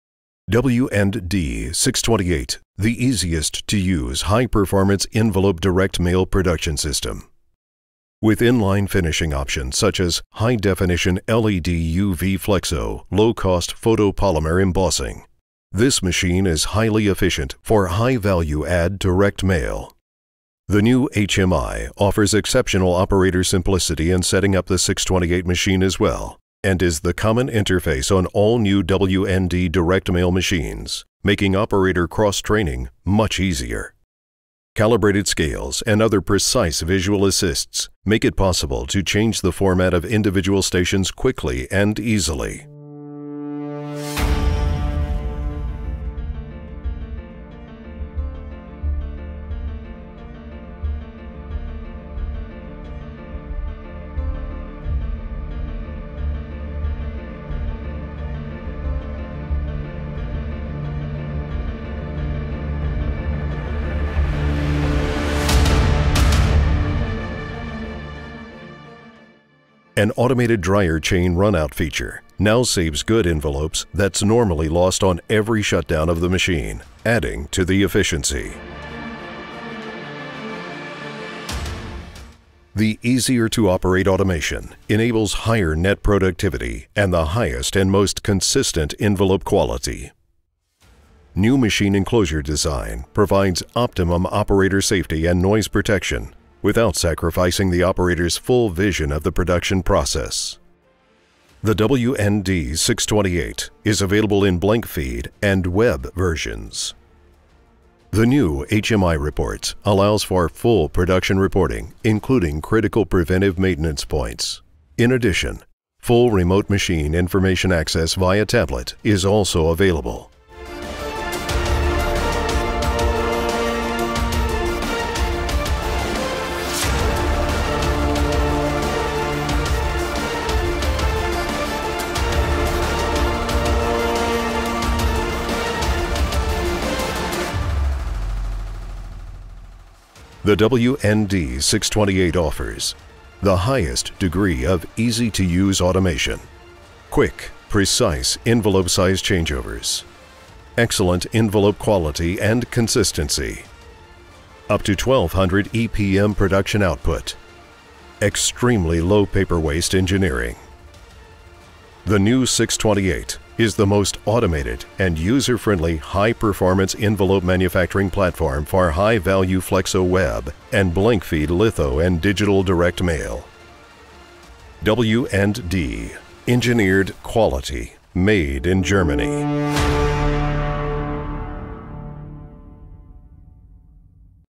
New W+D 628 envelope converting system (ENGLISH)
The W+D 628C is the latest high performance envelope machine (1200 epm) which was introduced at W+D Direct Mail Days this past June that includes the most advanced fold quality, waste and productively enhancements and easy to use adjuster interface specifically designed for high value pre-print direct mail.  The W+D 628 is available in both web and cut sheet configurations.